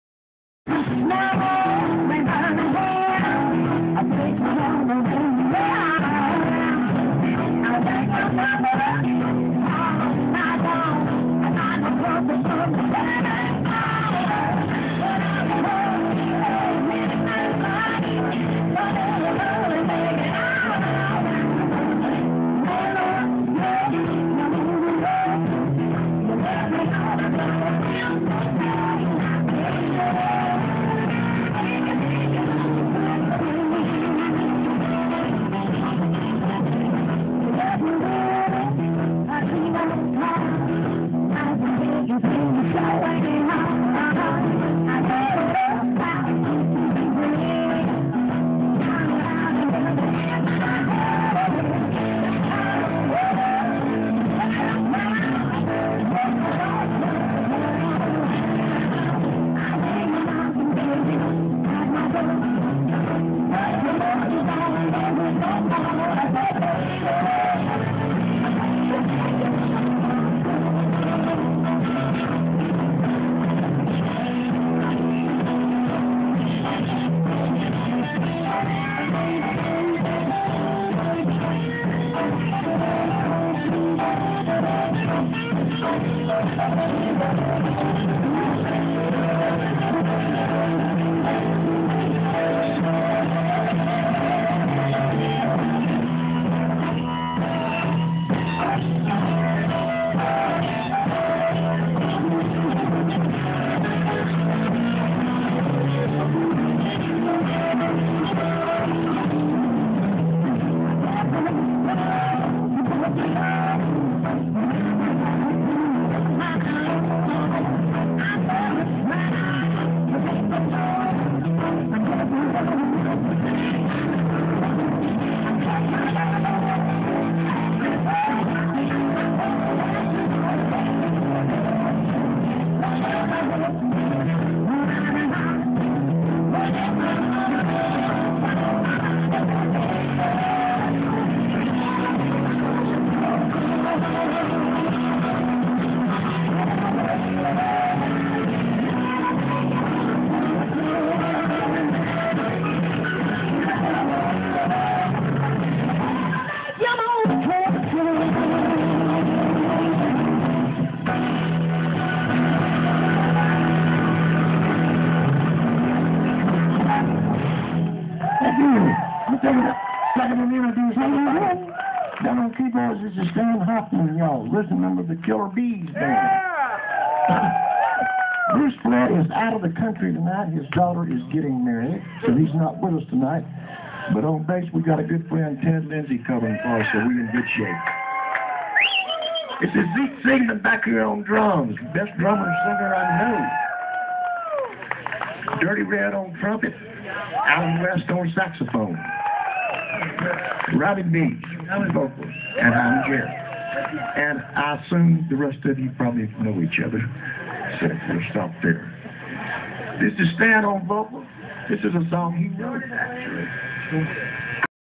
Robin And The Bluebirds Live 2012 Linden Texas
Robin And The Bluebirds Live 2012 Linden Texas, from the outdoor stage at Music City Texas.